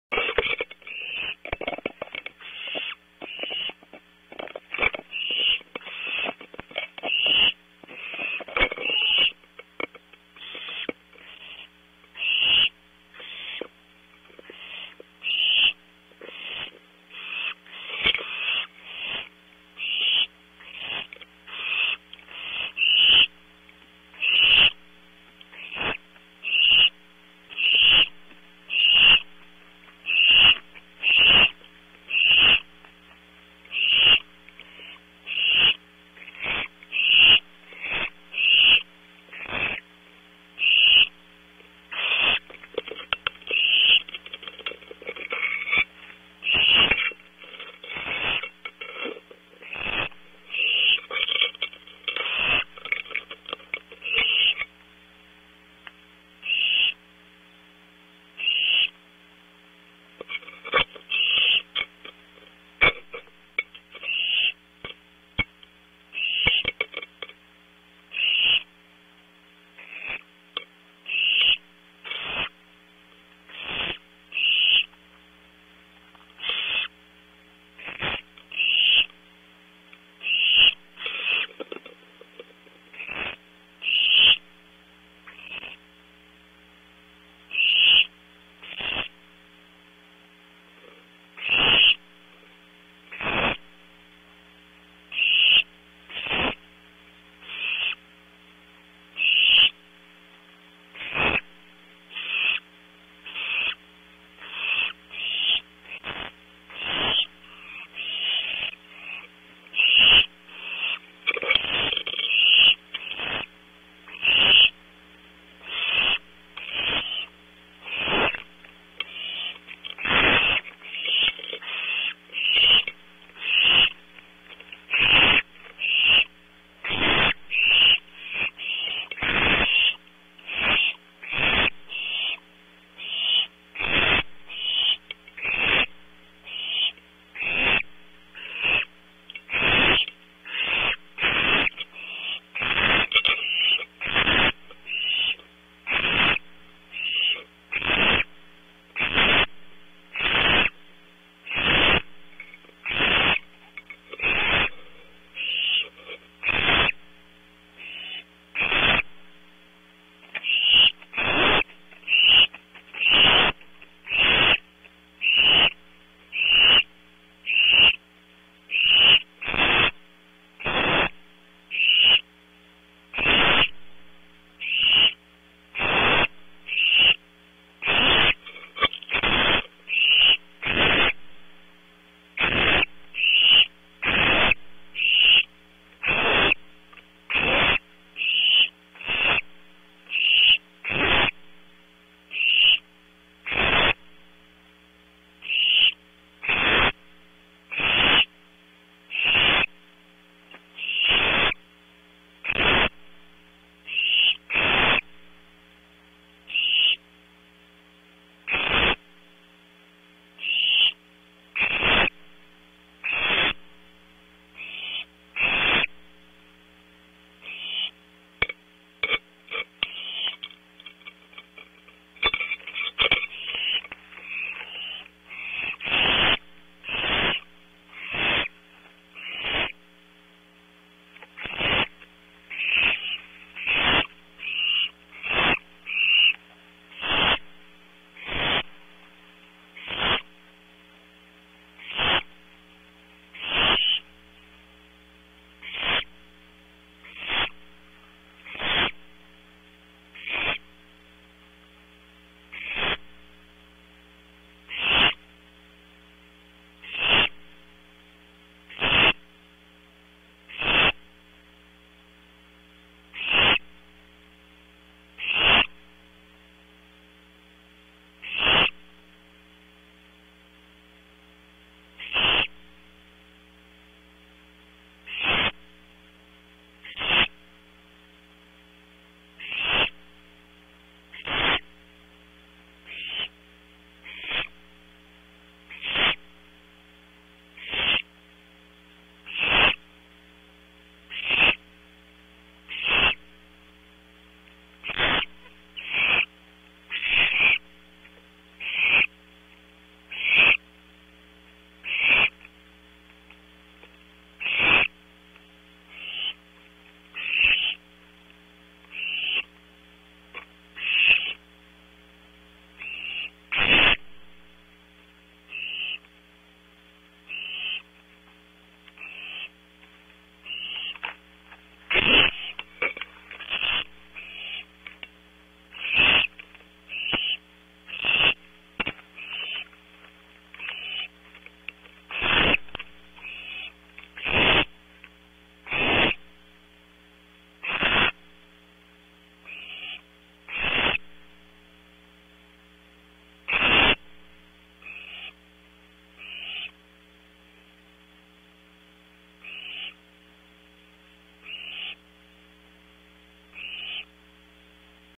Tonight's close-up PTZ view of owlets - Audubon Starr Ranch Barn Owls
6 cams on one page = Ritz cavity, Nestbox external, House Wren, House Finch, Kestrel Nestbox colonised by Honey Bees, Trail